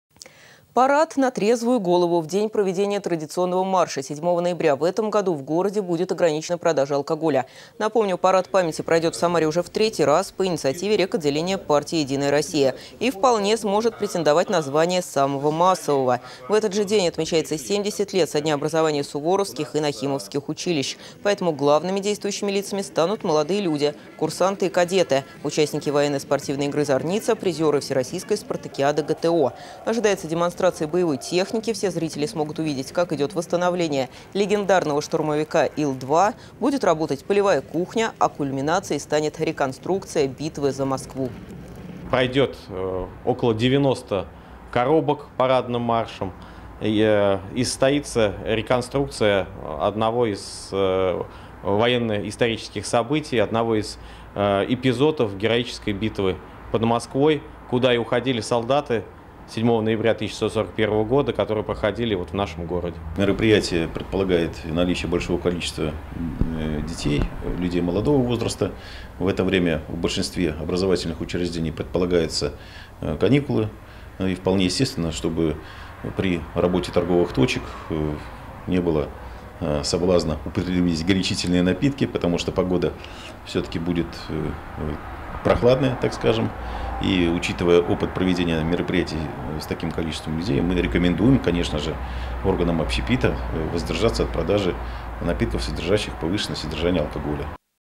7 ноября в Самаре будет ограничена продажа алкоголя
Парад на трезвую голову. В день проведения традиционного марша 7 ноября в этом году в городе будет ограничена продажа алкоголя. Напомню, парад памяти пройдет в Самаре уже в третий раз - по инициативе реготделения партии "Единая Россия". И вполне сможет претендовать на звание самого массового.  В этот же день отмечается  70 лет со дня образования суворовских и нахимовских училищ. Поэтому главными действующими лицами станут молодые люди, курсанты и кадеты, участники военно-спортивной игры "Зарница", призёры всероссийской спартакиады ГТО. Ожидается демонстрация боевой техники, все зрители смогут увидеть, как идет восстановление легендарного штурмовика ИЛ-2, будет работать полевая кухня, а кульминацией станет реконструкция битвы за Москву.